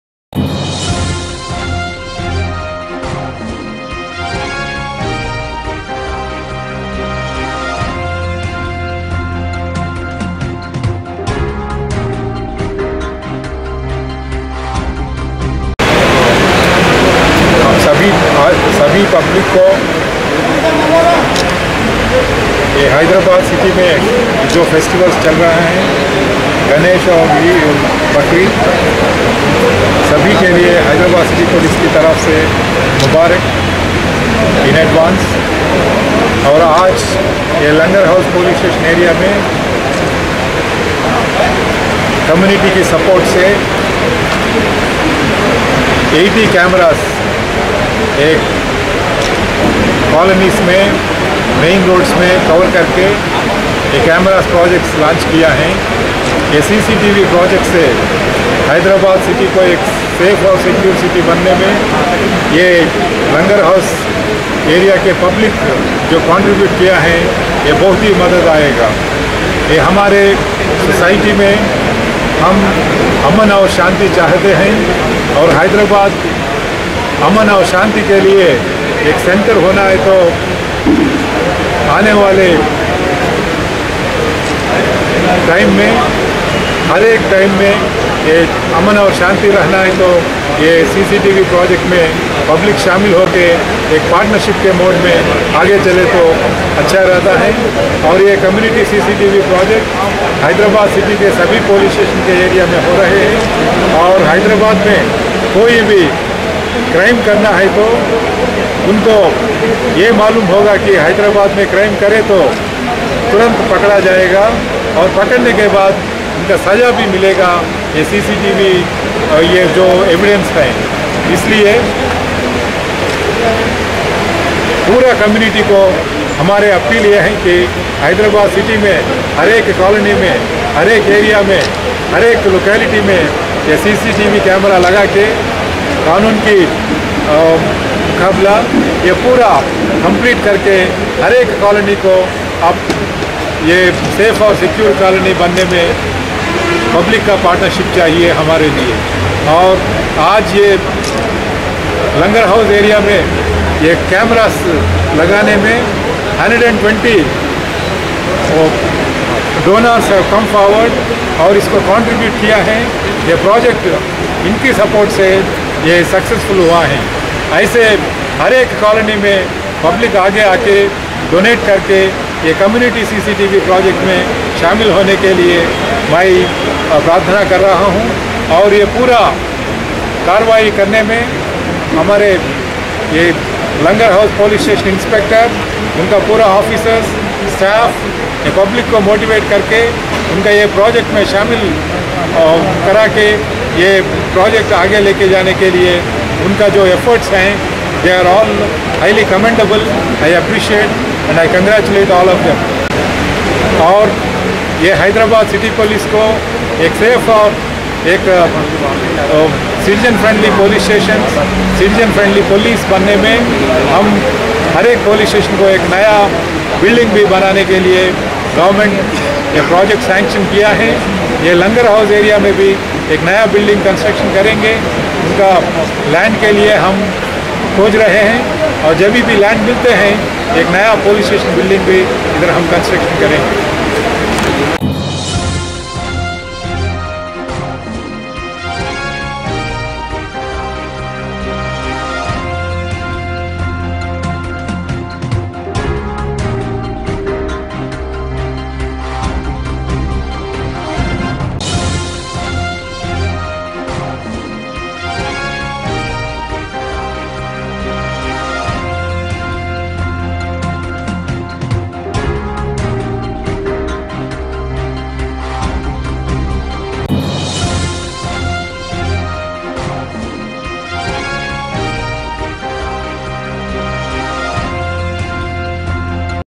There will be inauguration of 80 CCTV Cameras launching under Community CCTV project
There will be inauguration of  80 CCTV Cameras launching  under Community CCTV project by Sri.M.Mahendar Reddy,IPS, C.P.Hyderabad Today(8th September,2016) at 4.30pm at D-FORT Function Hall near Langer House x road, Langer house P.S limits.